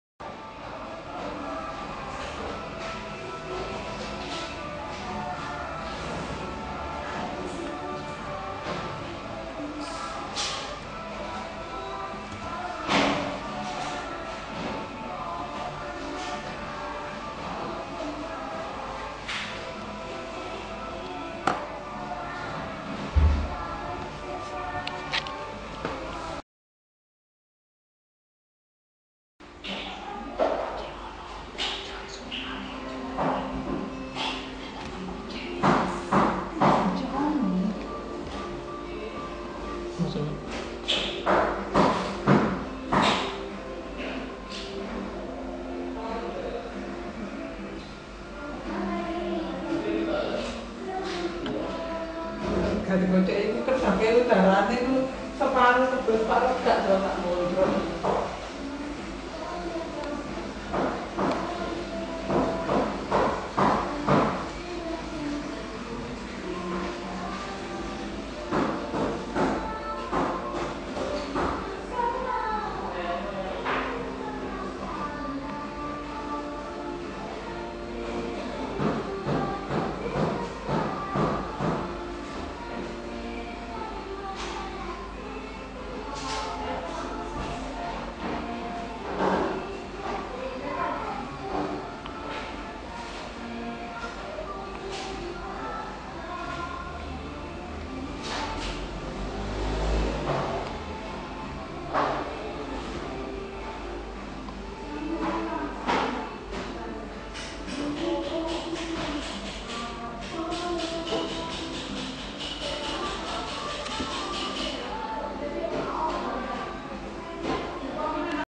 How to fill the ink from the tube to the cartridge canon iP4500
Printer Maintenance | CANON IP4500